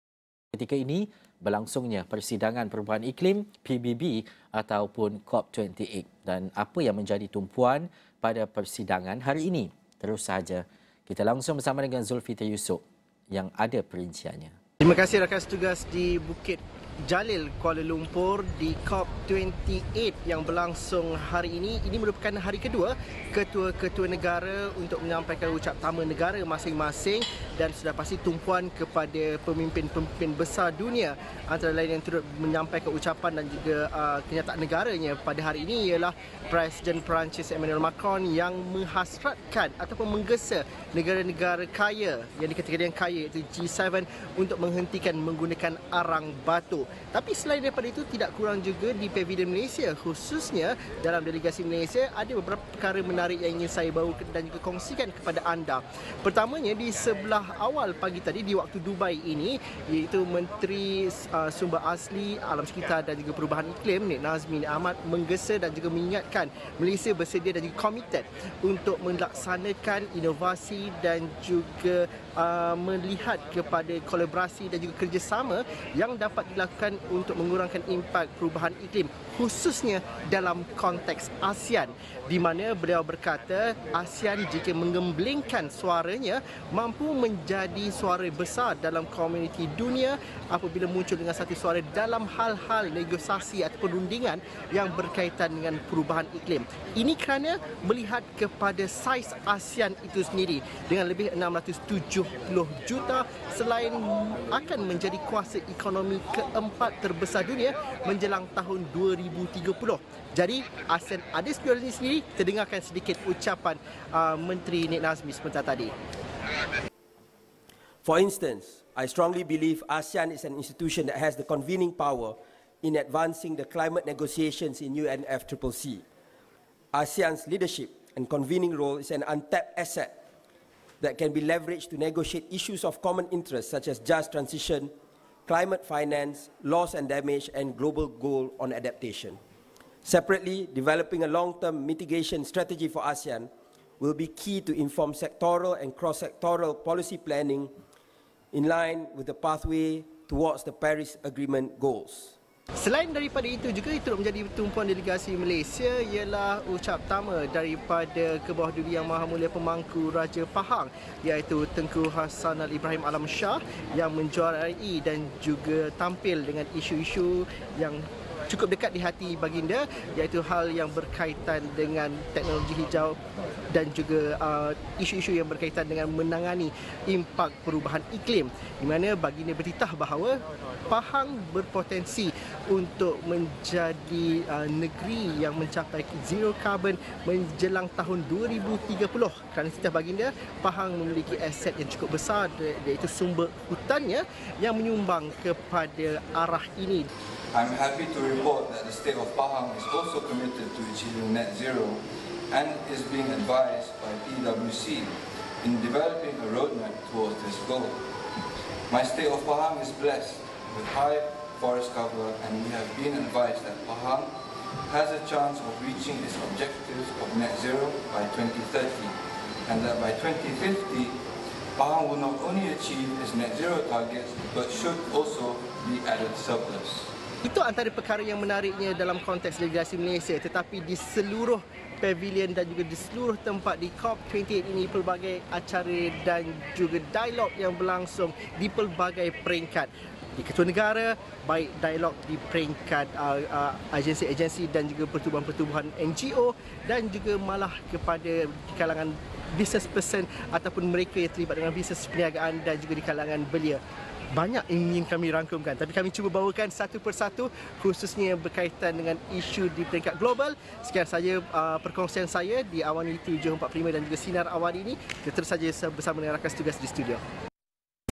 COP28: Malaysia komited dukung pelaksanaan pelan tindakan iklim
Editor Astro AWANI, Dzulfitri Yusop yang membuat liputan Persidangan Perubahan Iklim PBB (Cop28) membawakan perkembangan terkini dari Dubai.